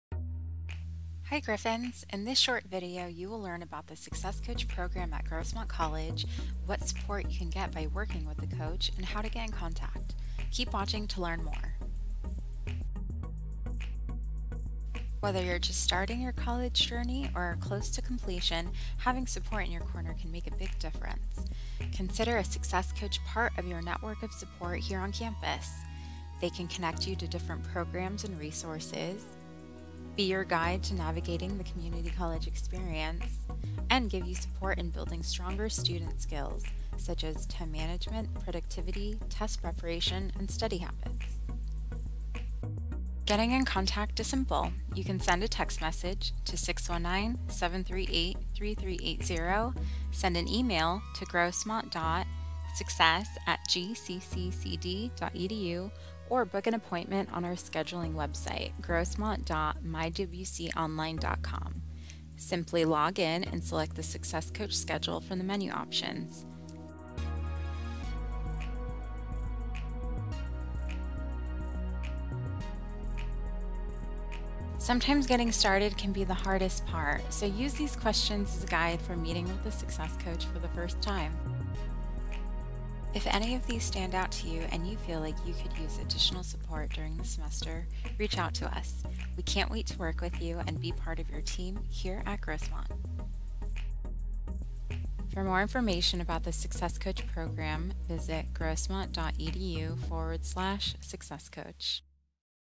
Success Coach Program at Grossmont College
This short video introduces the Success Coach program at Grossmont College and what supports are available for students.

Date created: Summer 2022
Video length: 1 minute 42 seconds